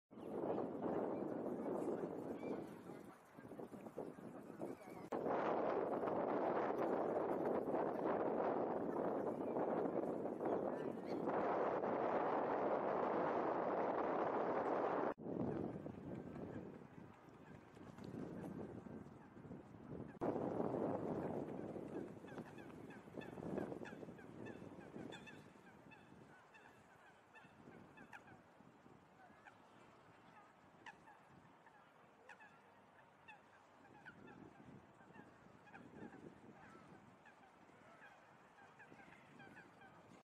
Pied Wagtail pair beside the Serpentine
A pair of Pied Wagtails hunt on the grass beside the Serpentine and along the shore. They keep a distance from each other to avoid collisions when they suddenly dash after insects. But if you stand still, sometimes they will come close to you.